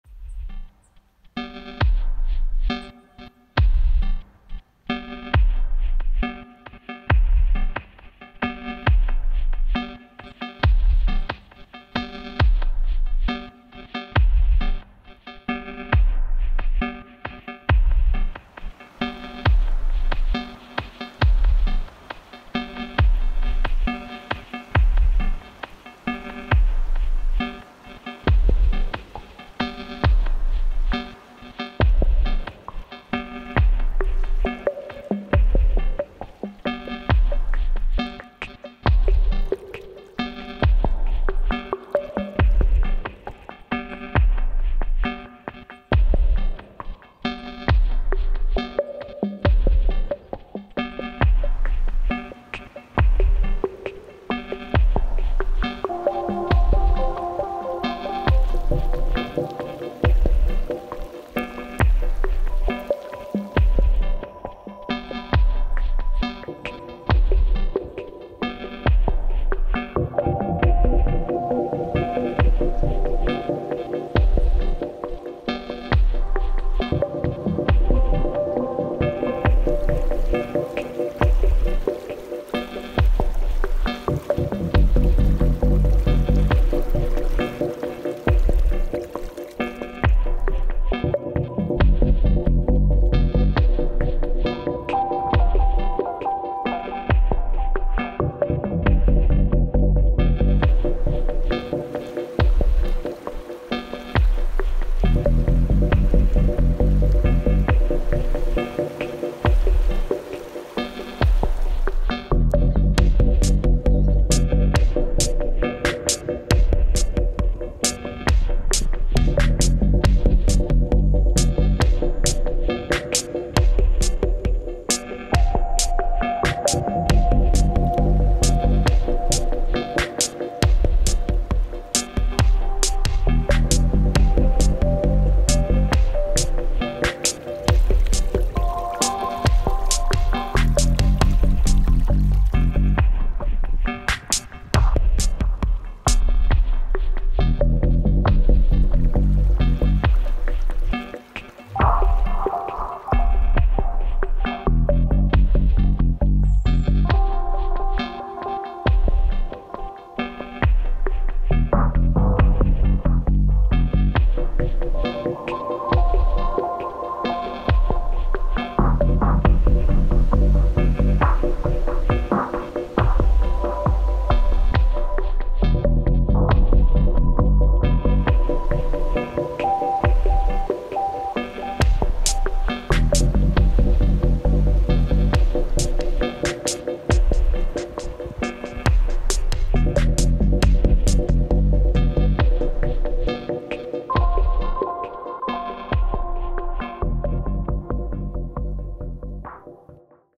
[4K] Teshio river Hokkaido 天塩川 / slow fishing
2020年10月3&4日 天塩川 (Teshio river Hokkaido)

イタドリの葉が山吹色に色付き始めるなど、フィールドには少しずつ秋の気配が近づいていた。
稲刈りも始まり、夏から秋へとゆっくりとシフトしているようだ。

Camera & Edit : iPhone7 & iMovie